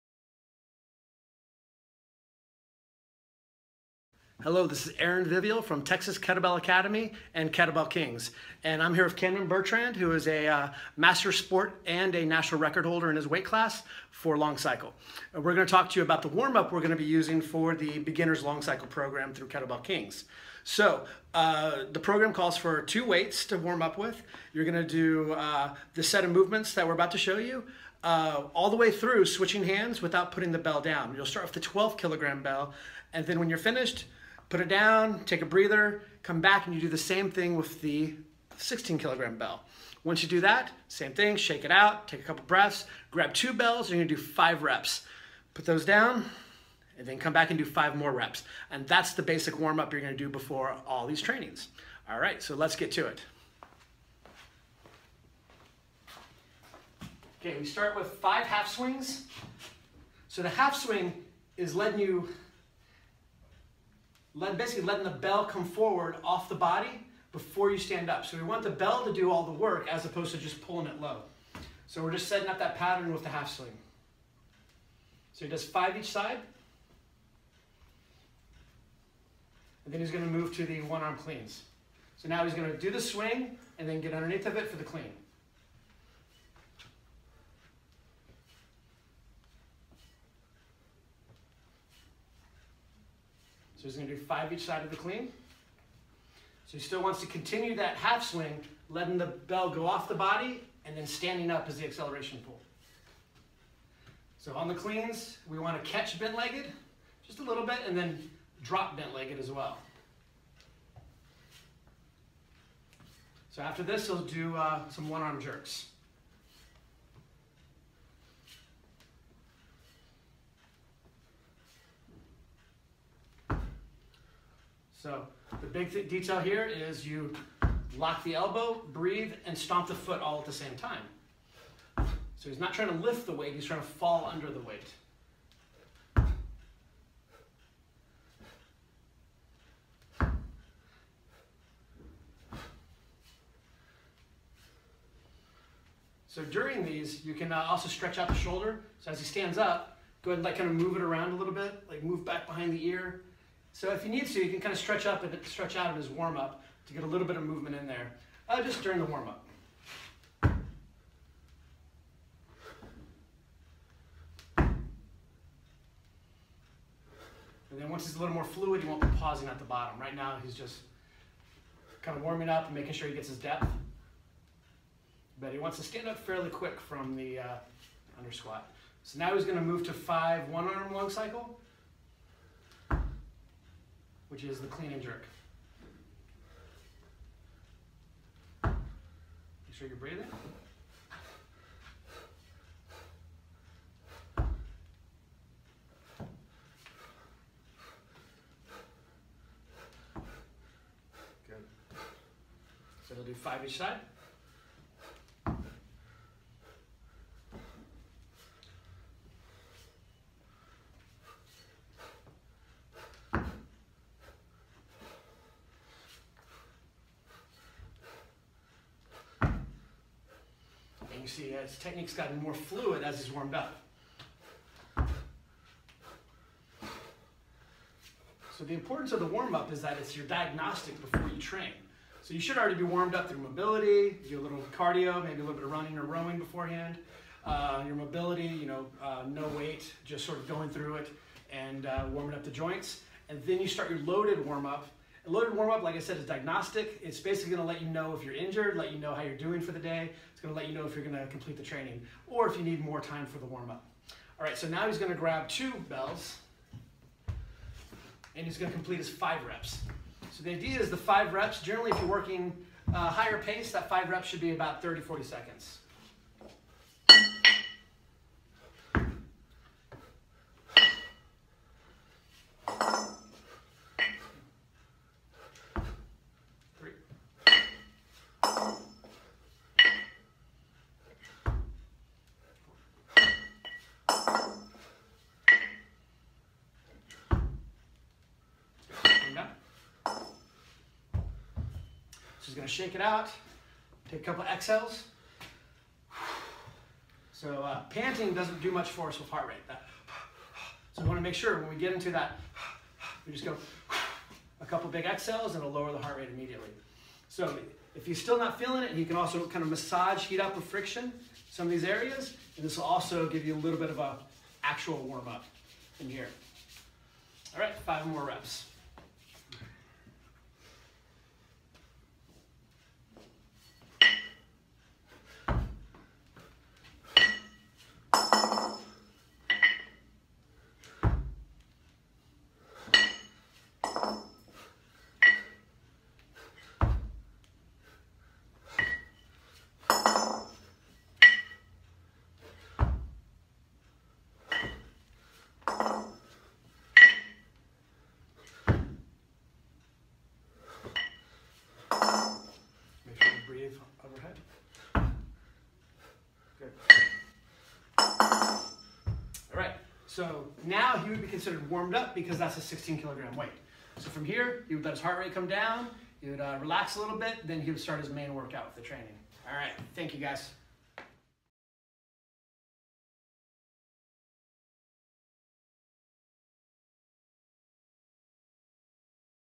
Kettlebell Kings Presents: Warm Up Demo for Our 4 Week Intro To Kettlebell Sport Training Program
Kettlebell Kings has created a 4 Week Beginner Training Program for Kettlebell Sport. This breaks down one of the movements in the training. Kettlebell Sport has all the elements that make for a good training regimen and workout! You build strength, endurance and test your will!

The Basics of Kettlebell Sport:

1) endurance kettlebell lifting, lifters have 10 minutes to complete as many reps as possible
2) efficiency and technique are important to maintain energy throughout set
3) lifters compete in specific lifts with either one arm or two arms

The Benefits:

1) great goals to motivate your workouts
2) blend of strength, endurance, balance and coordination in one workout equals efficiency
3) build amazing mental tenacity, this carries over to other aspects of life